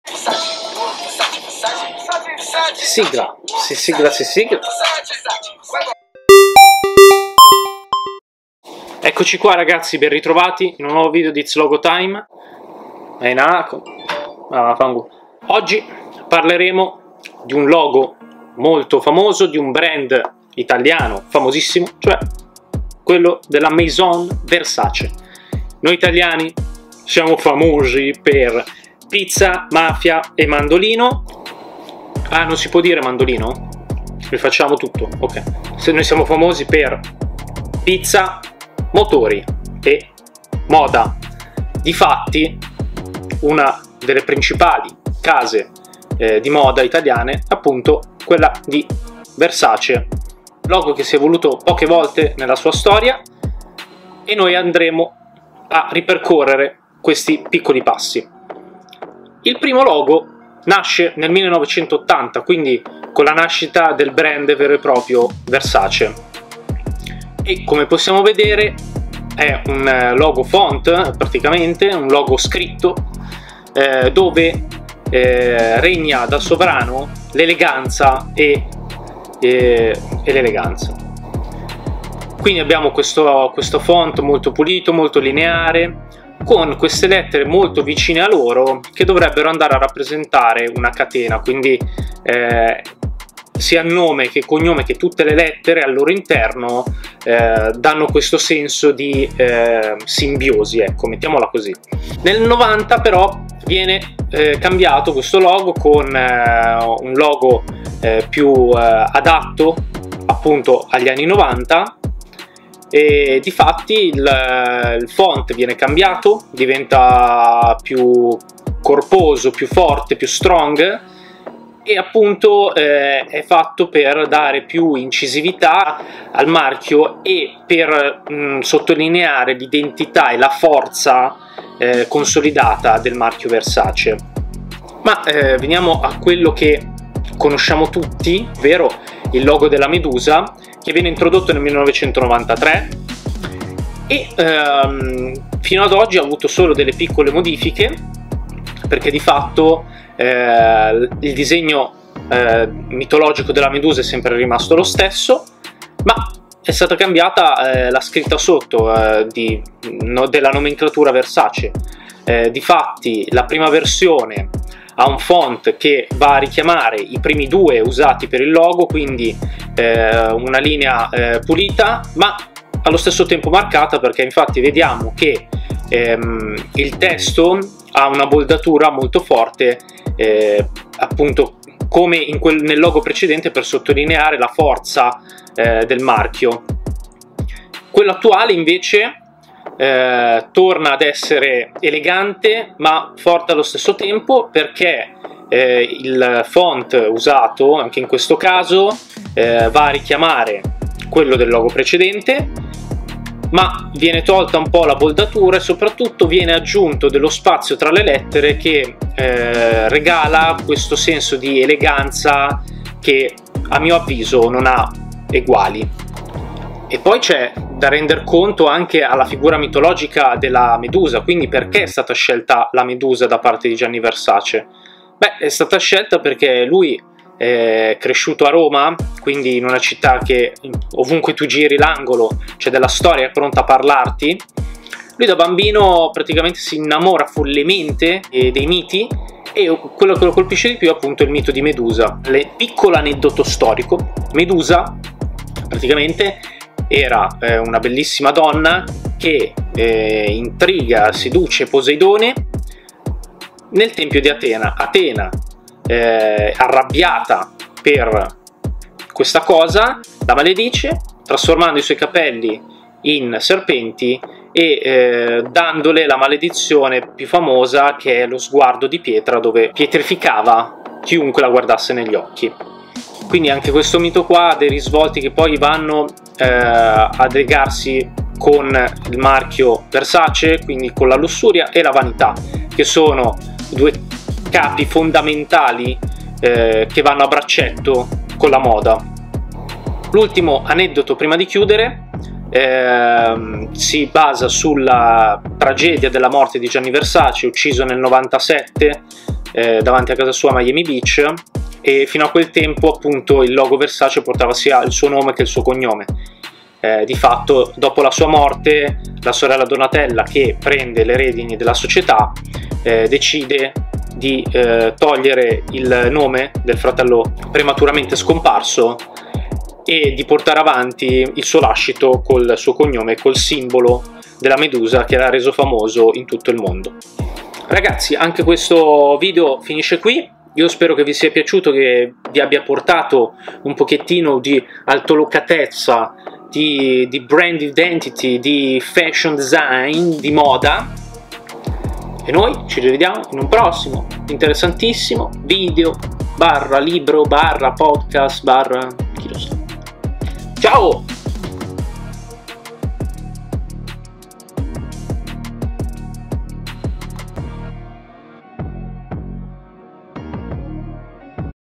VERSACE - IT'S LOGO TIME EP.12
Ripercorriamo la storia e l'evoluzione del marchio di moda italiano VERSACE.

E come al solito/and as usual: Enjoy the logos!